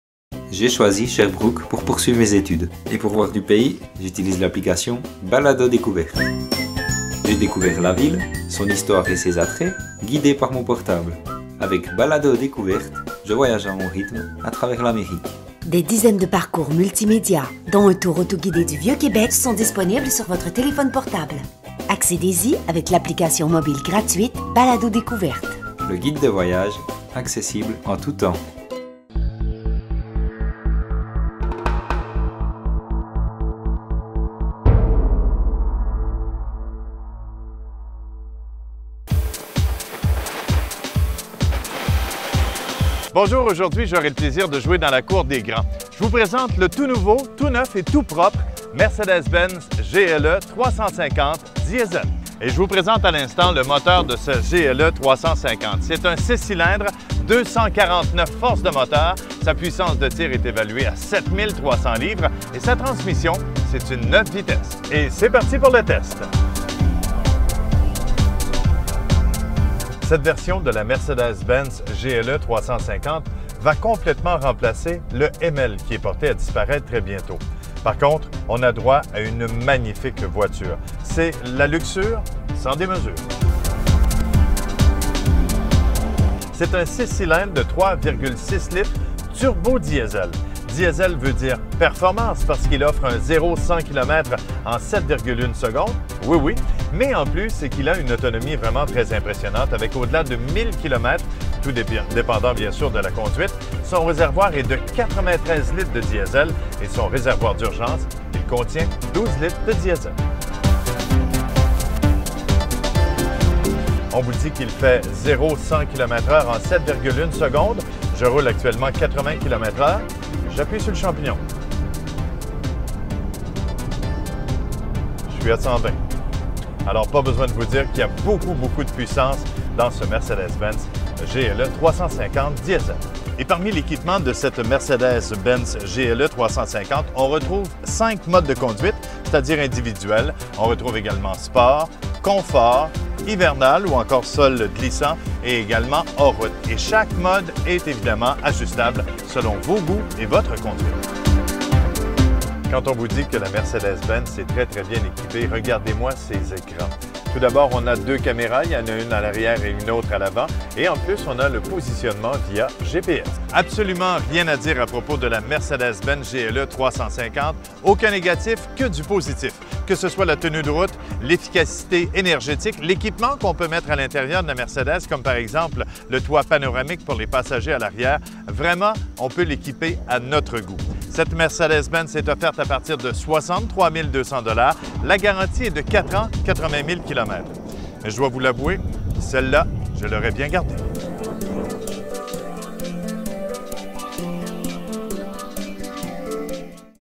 Mercedes, le VUS GLE 350 D - EstriePlus.com
J'étais vraiment excité d'essayer le tout nouveau modèle de Mercedes, le VUS GLE 350 D. Un modèle qui remplace le ML qui ne sera plus disponible.